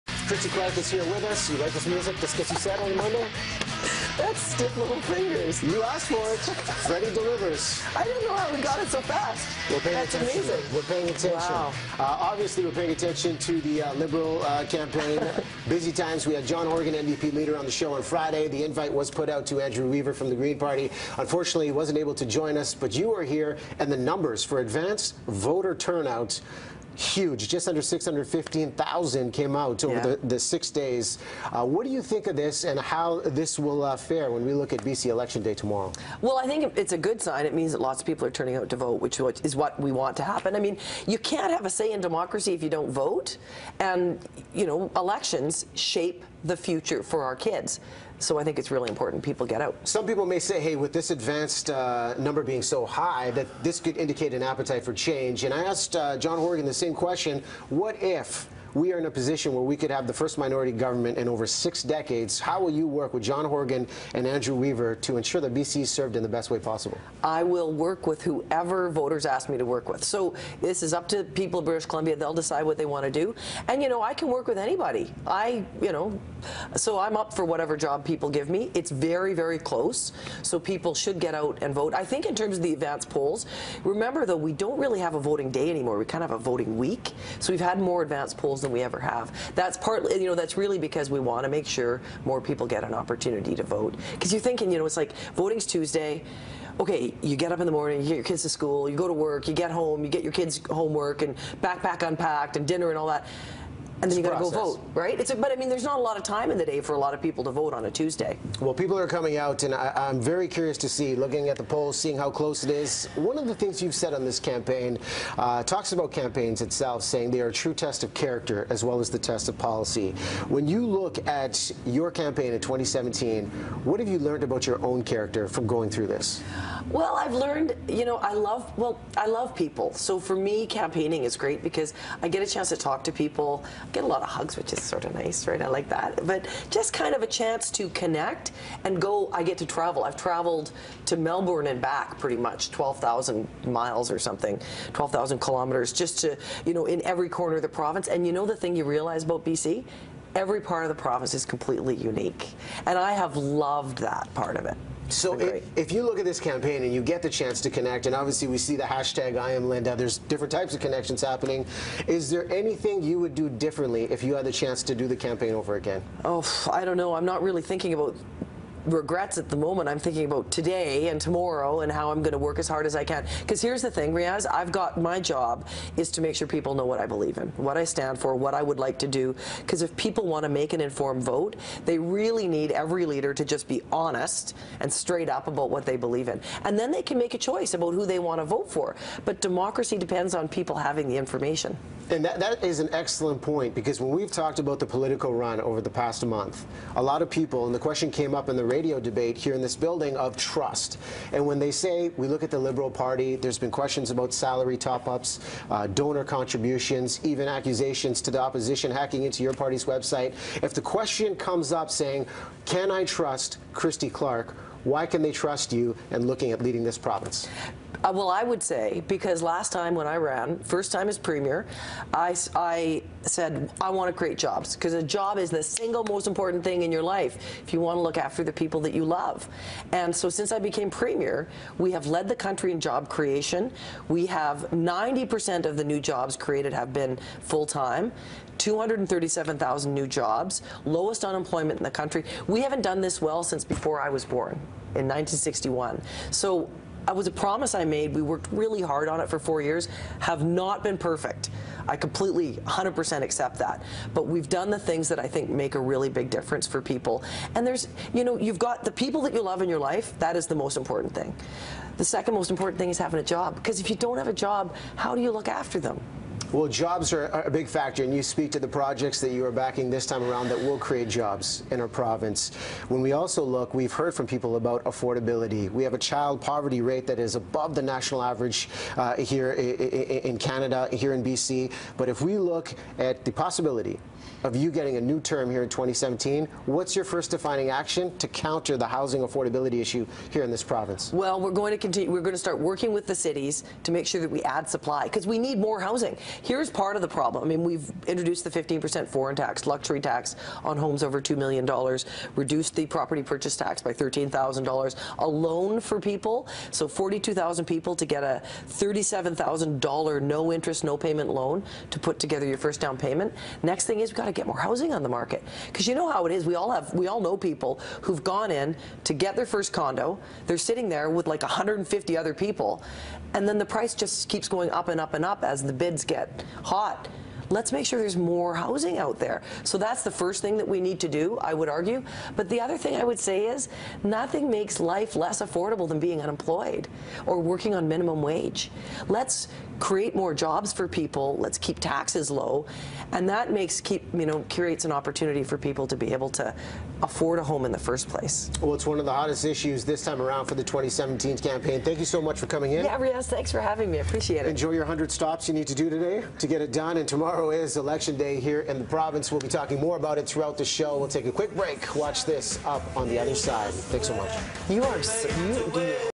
Why Christy Clark says voters can trust her
The final countdown is on in the 2017 BC Election campaign. Liberal Leader Christy Clark sits down on the BT couch one more time before you head to the polls.